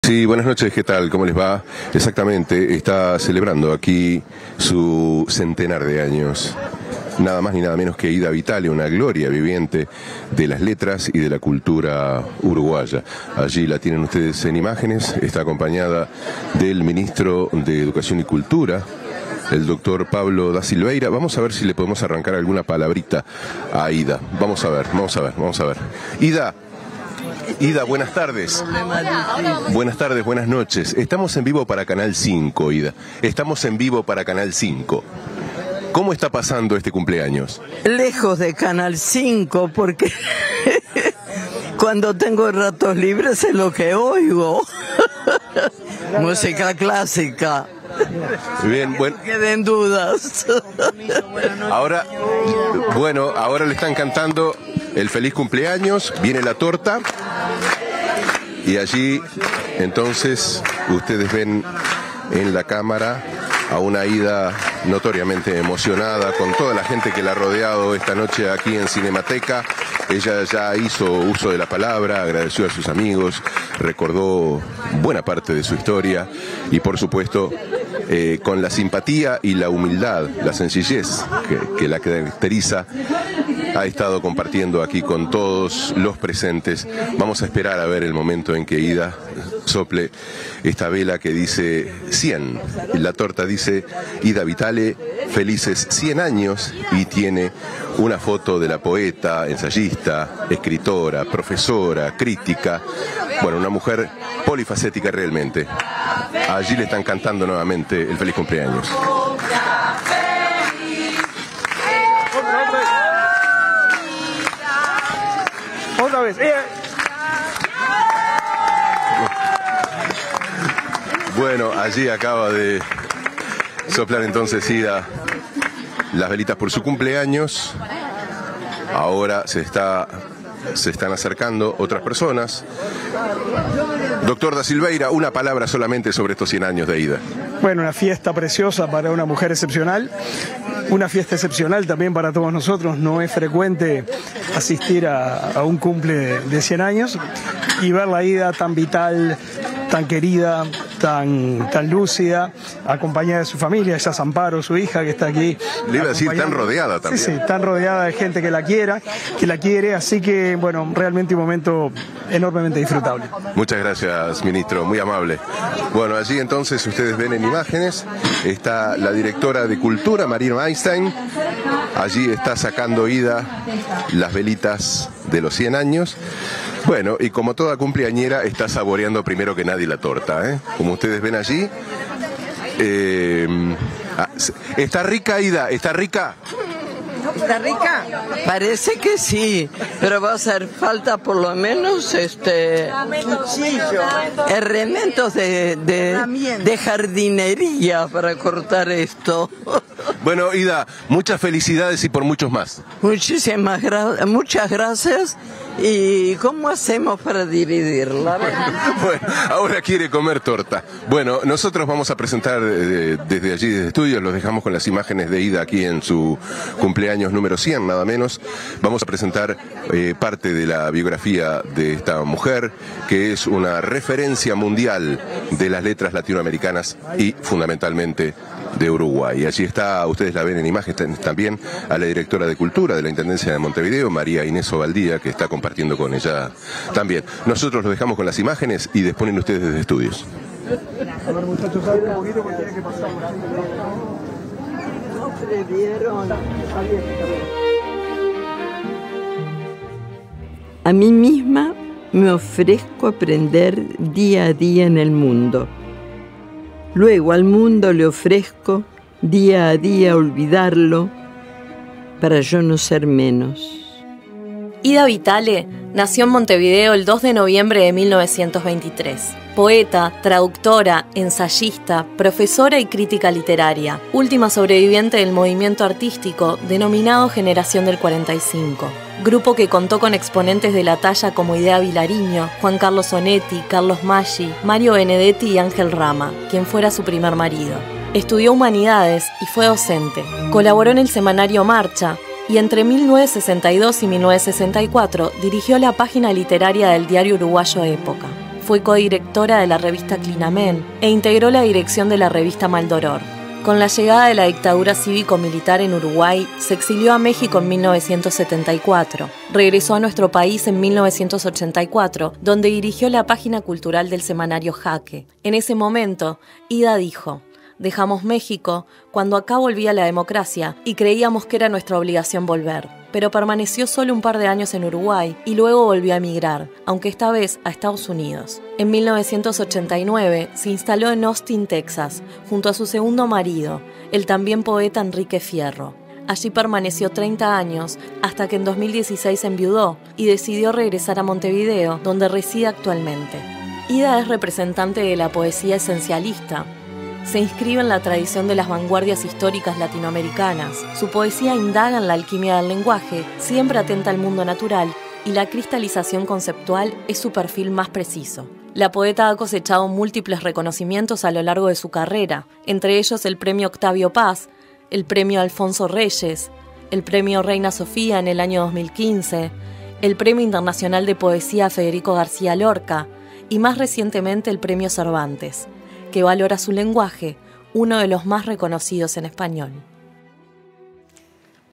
Se festejan los 100 años de Ida Vitale en Cinemateca, con la presencia de la poeta
Informe de Camilo Delelli